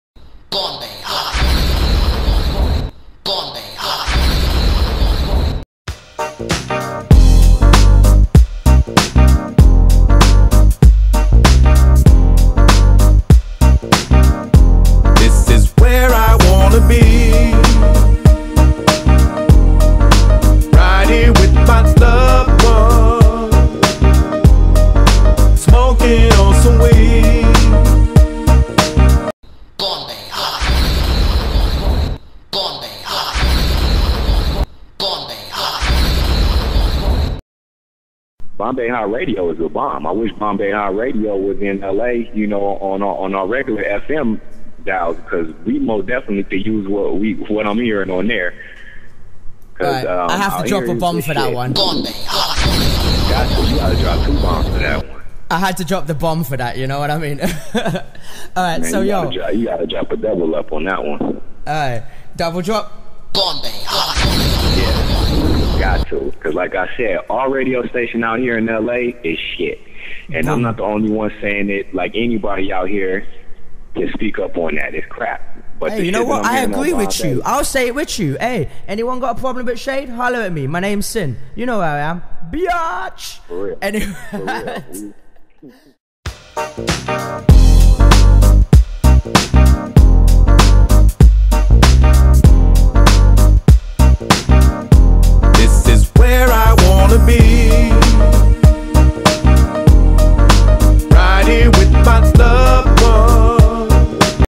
DJ SIN INTERVIEWS SHADE SHEIST about www bombayhottradio com -
DJ SIN interviews Shade Sheist from LA where Shade talks about